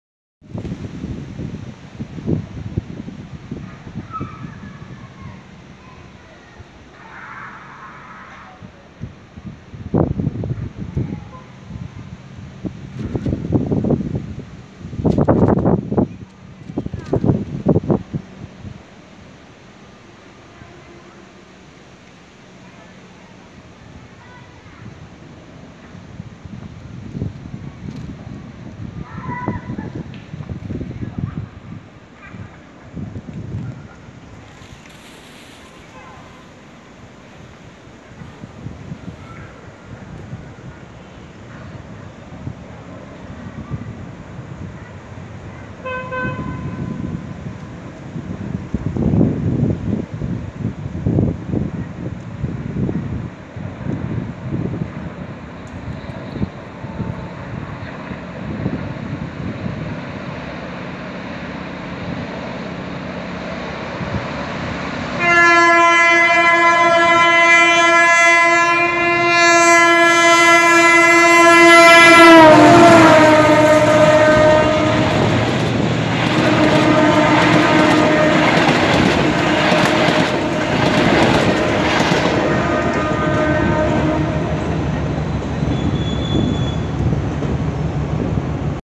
La Croata volando por Villa del Parque!
La G26 1000, al mando del tren a Junín del día 23/11/2021, hace su muy veloz paso por la estación Villa del Parque levantando más polvo que el viento!